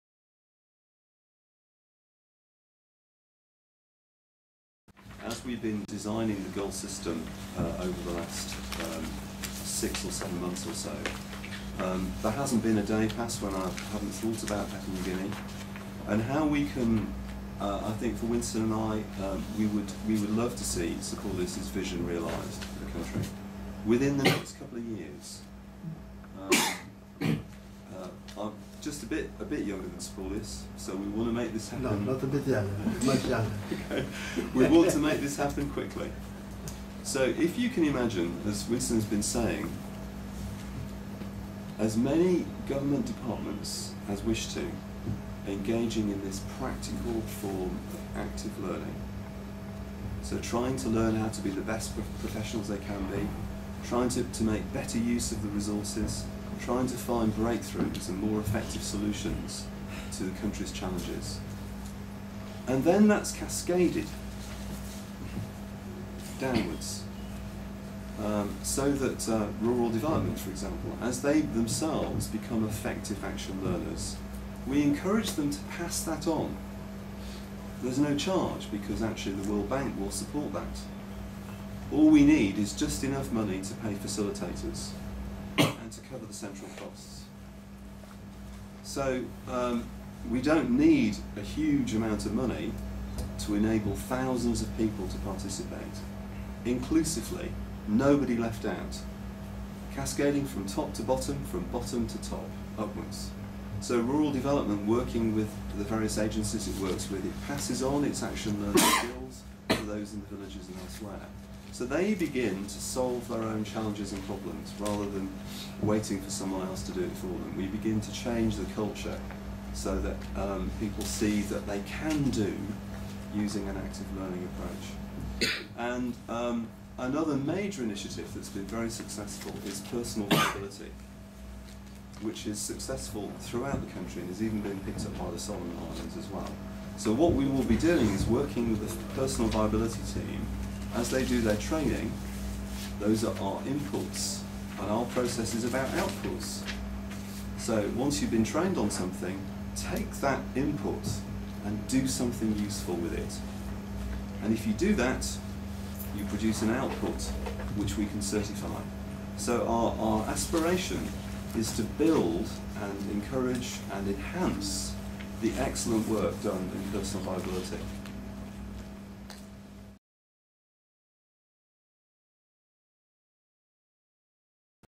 10. Next steps in PNG | Press Conference | Inauguration | GULL Media
Dr Richard Teare, President, GULL / Mr Winston Jacob, GULL Country Representative, Papua New Guinea.

Our ambition is to move quickly and to enable Sir Paulias' vision for the people and Nation of PNG to be realized within the next few years. The goal is an inclusive one - anyone in PNG will be able to participate in GULL.

In this 10 part series the press conference will address GULL's global and national objectives: A briefing to the news, TV and radio media of Papua New Guinea.